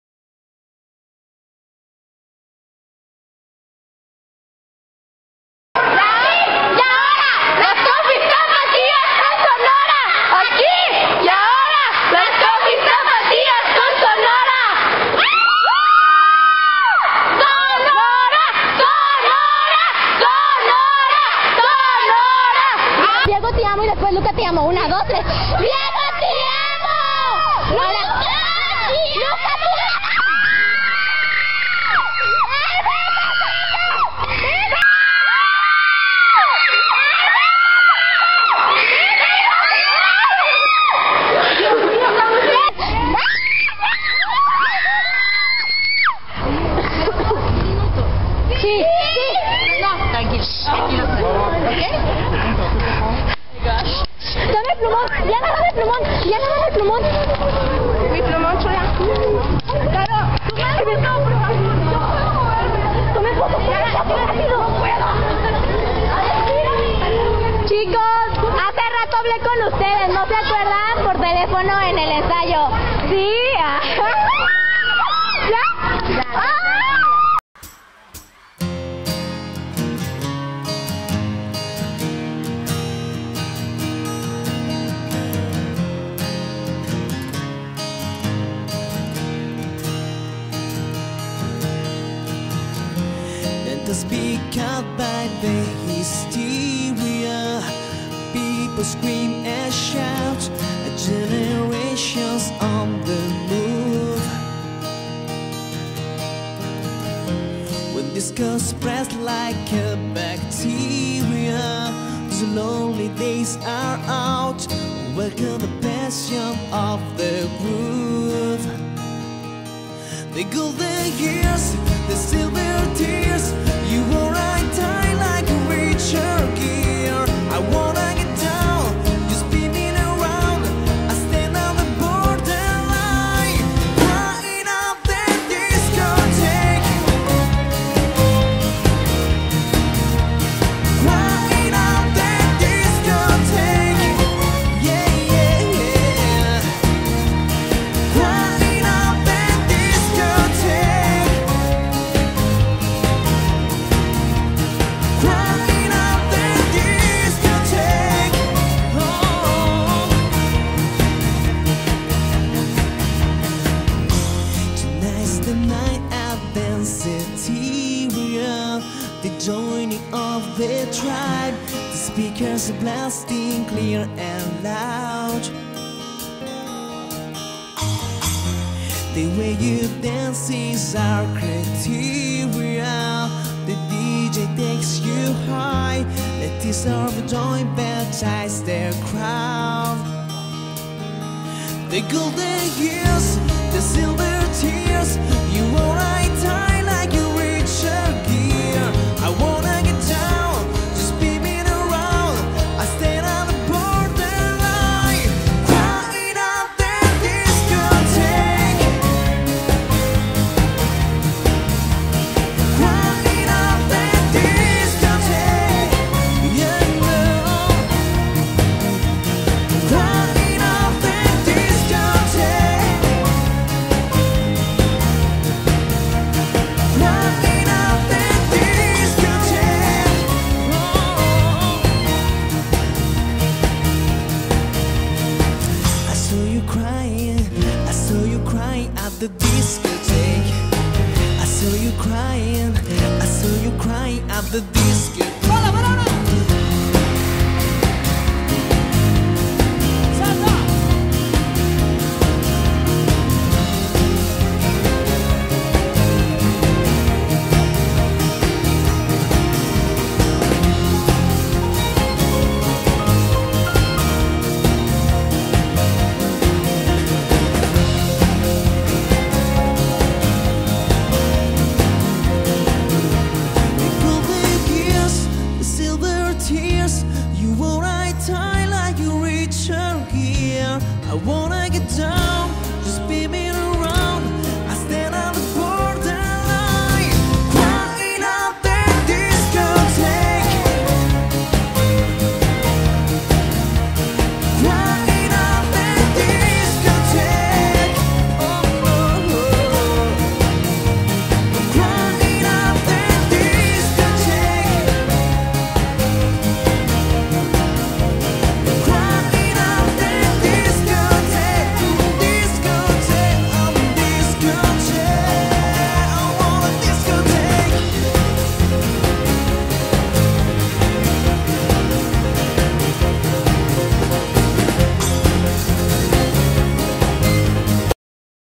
Sonohra en Guadalajara por 2°vez - 1 parte
hola aqui les dejo este video de sonohra cuando estuvo en GDL
por segunda vez espero que les guste
esta es  la primera parte despues subiremos las demas

feliz dia internacional de Sonohra!!

aqui y ahora las Cofis tapatias con Sonohra!